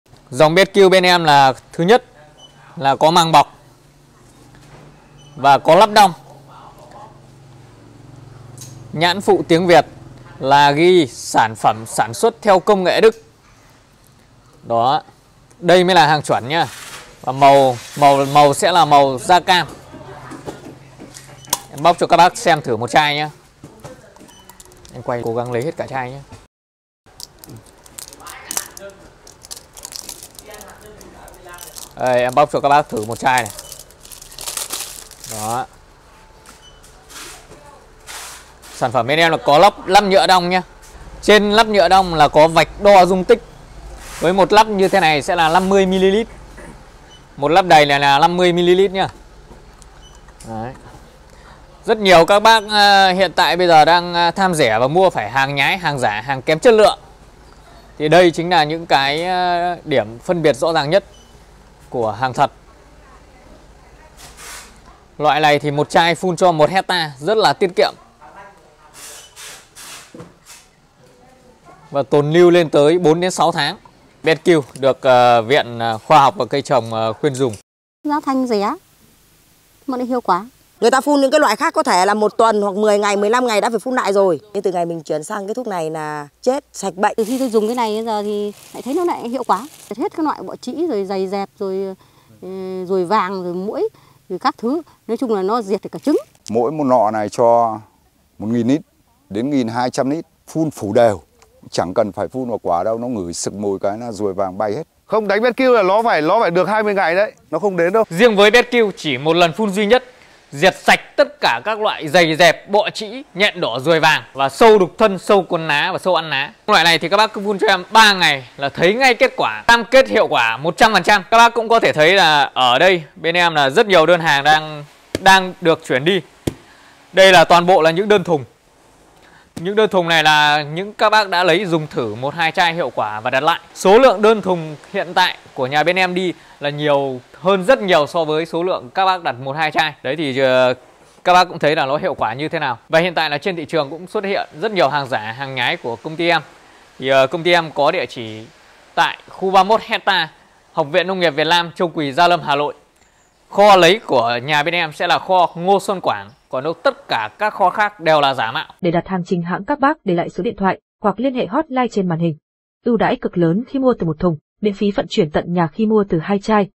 Cách phân biệt thuốc diệt côn trùng BESTKILL thật và giả
NÔNG NGHIỆP KINH BẮC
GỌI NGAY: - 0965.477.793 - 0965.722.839
XEM GIÁ TẠI ĐÂY: